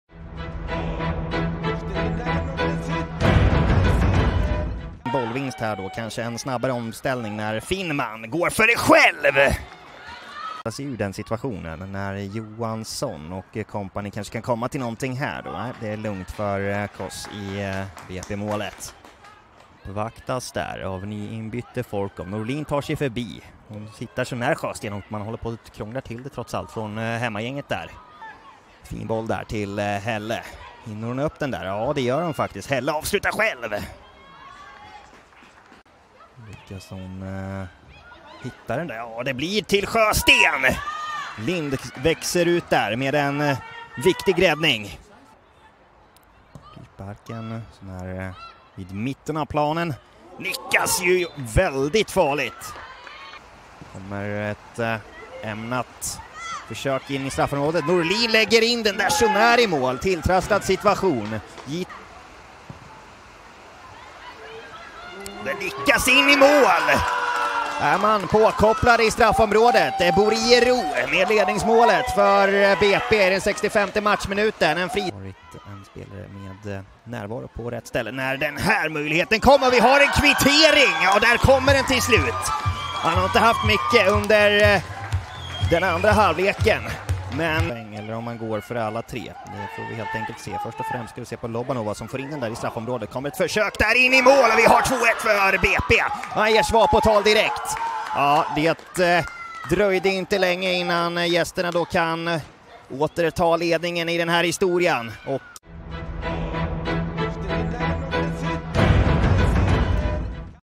Höjdpunkter: Jitex Mölndal BK - BP
BP tog en nödvändig seger i årets näst sista match i Elitettan mot Jitex Mölndal BK (6/11).
BP vann med 2-1 efter mål av Frida Boriero och Klara Andrup.

Se alla BP:s matcher i Elitettan på Sportexpressen Play.